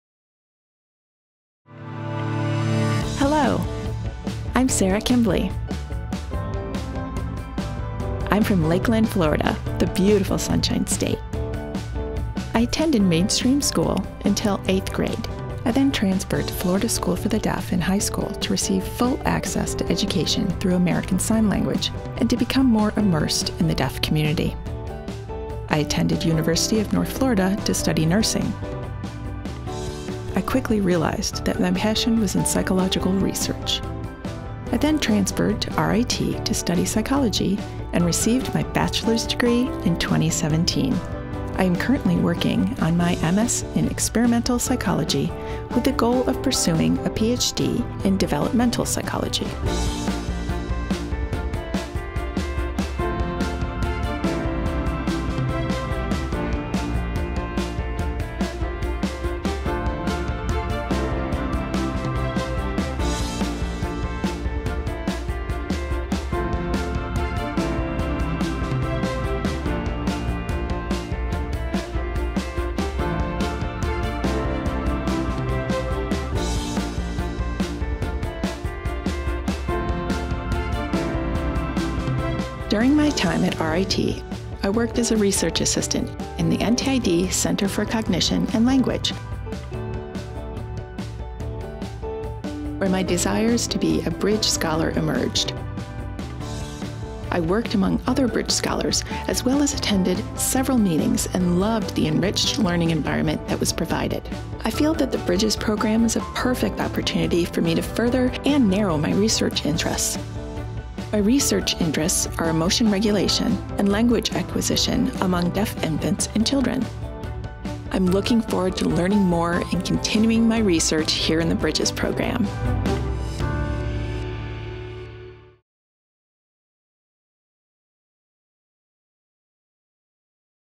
NTID Bridges Scholar: Sara Kimbley
If you are Deaf or Hard of Hearing and aspire to become a scientist, the Rochester Bridges to the Doctorate Program may be the perfect solution for you! or email